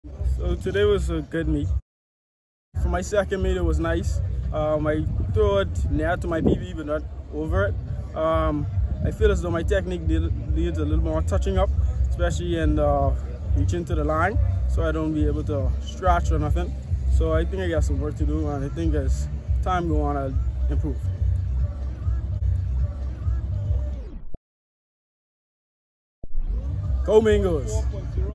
Freshmen Rashad Evan competes at Island Relay
Track & field specialist Rashad Evans finishes 8th in Javelin at IslandRelays with a best throw of 38.55m (126-6).